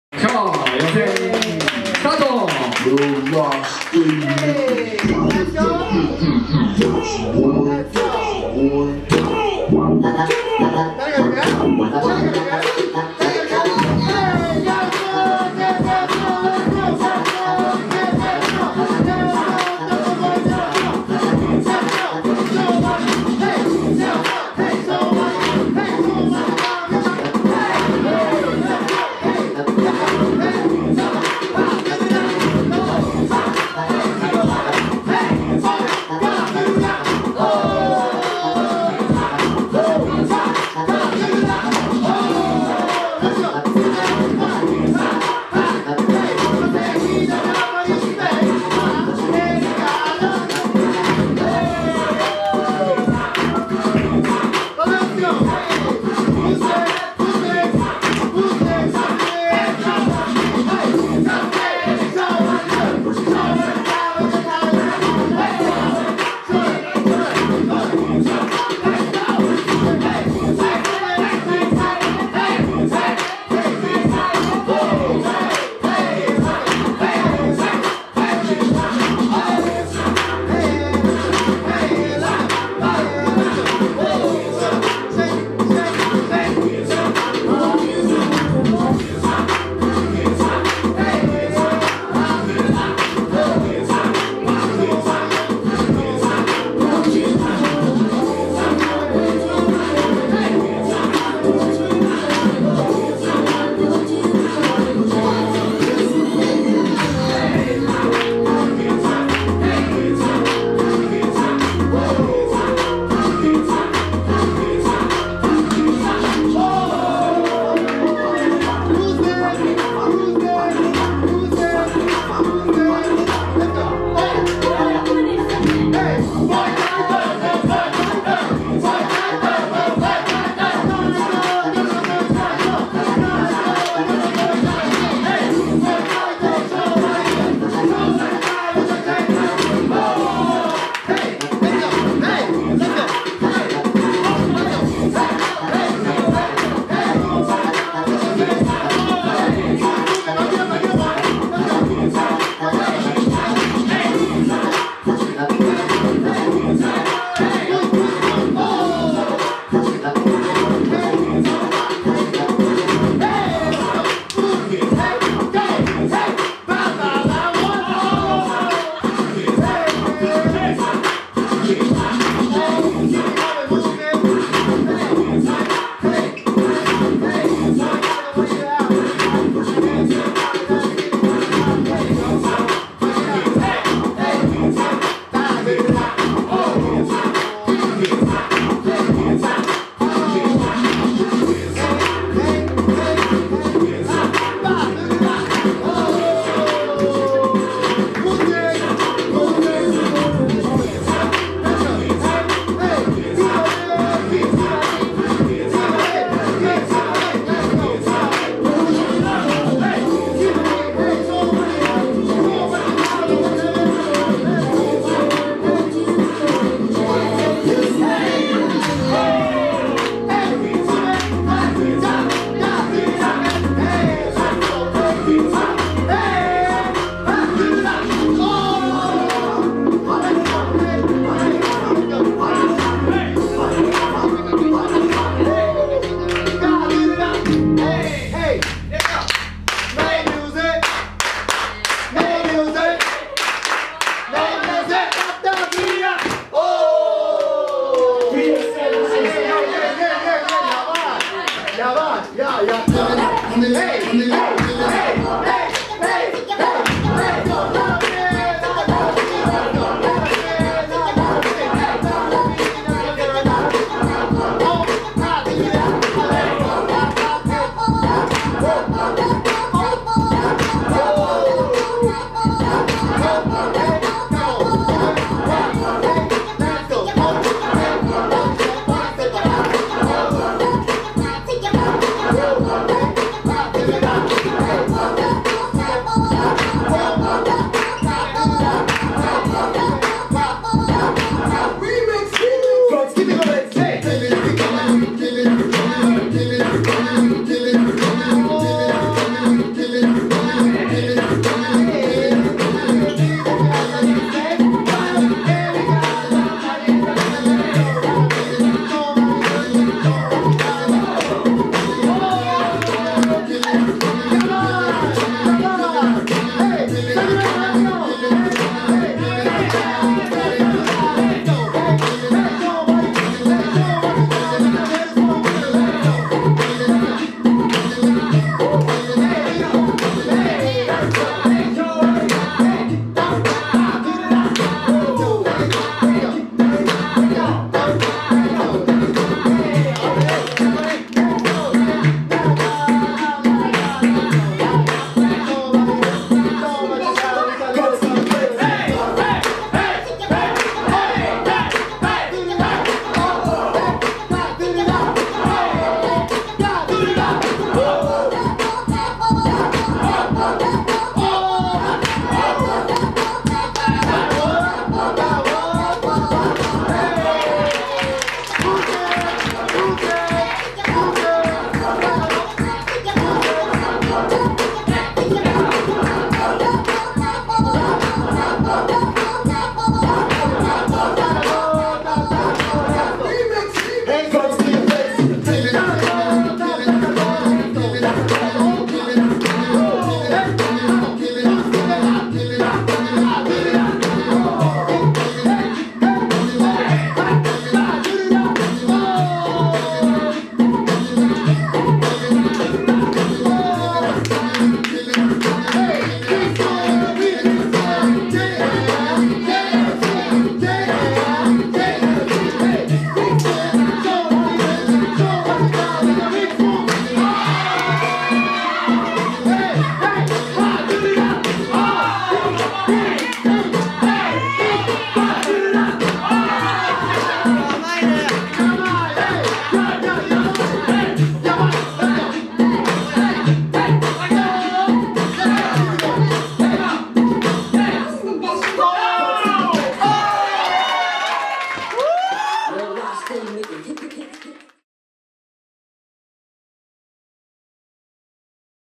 Litefeet Battle Japan Tokyo Side | 6.21.2014
XYZBOYZ presents Lite Feet Nation Battle at Japan Tokyo Side.
Entry List:
Nelson ( Ceo Nelson Crew
Kasmine
Boy Kaito ( Jumi Lite Club aka mini WAFFLE
Himari ( Level Up Crew
Nana-P ( 044Junior / Level Up Crew
Sakurako ( Nelson Crew
Nanaka ( Nelson Crew
Takato ( Level Up Crew
Riku Smoove ( Jumi Lite Club aka mini WAFFLE

Video by : XYZvisions